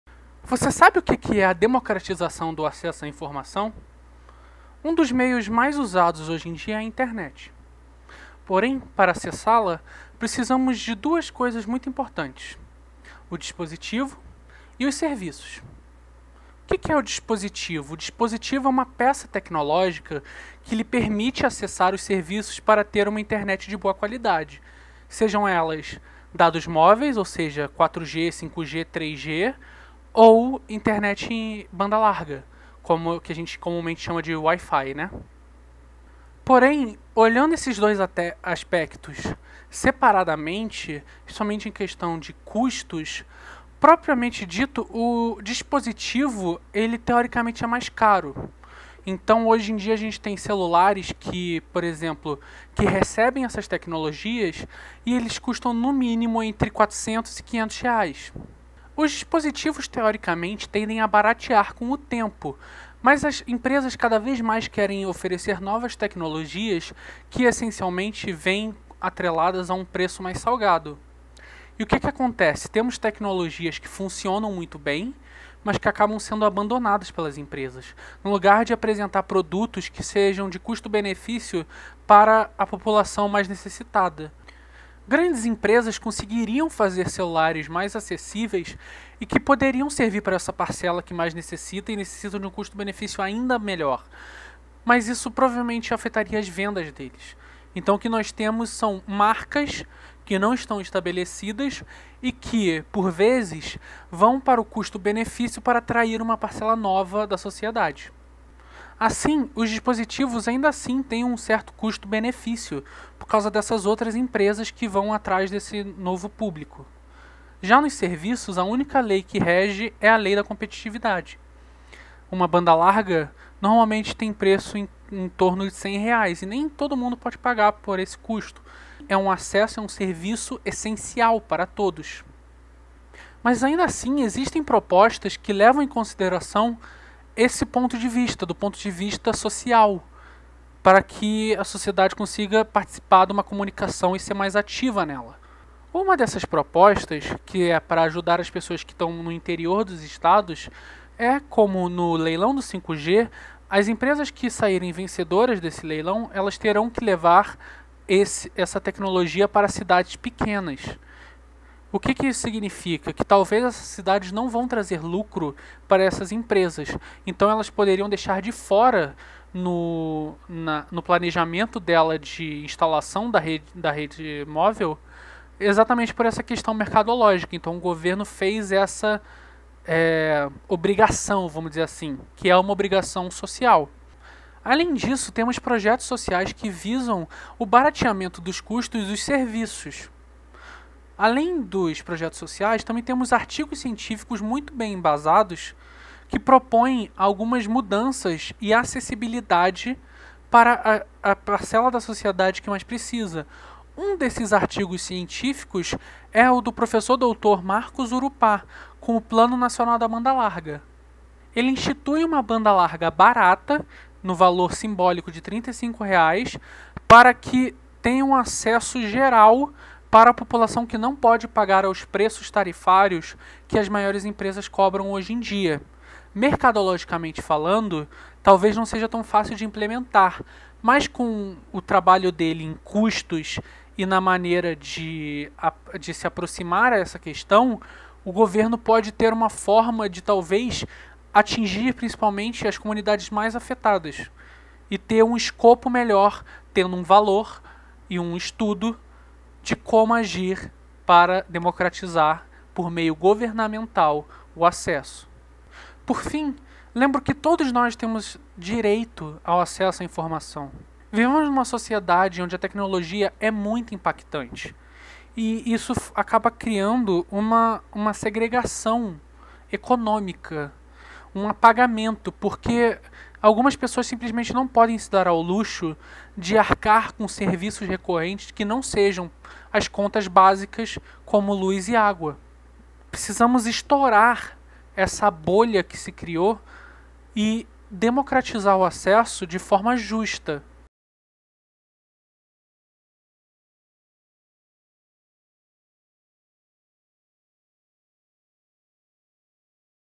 Trabalho final Comunicação e Sociedade
Trabalho realizado para a disciplina comunicação e sociedade, ministrada pelo professor Marcos Urupá, no semestre 1/21 da Universidade de Brasilia